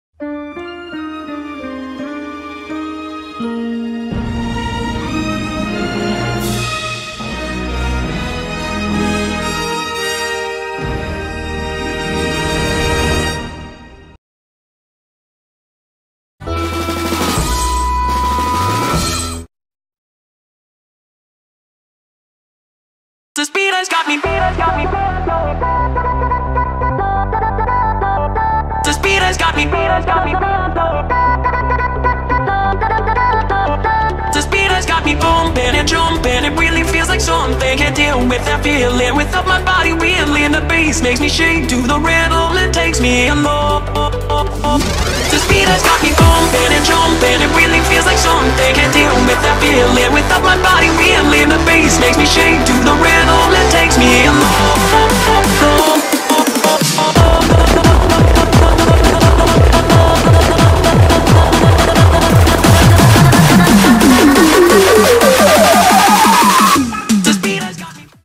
Fan-Made Logo Opening for a movie adaptation of Material Girl
A gift I made for Heartgear (the author of “Material Girl”).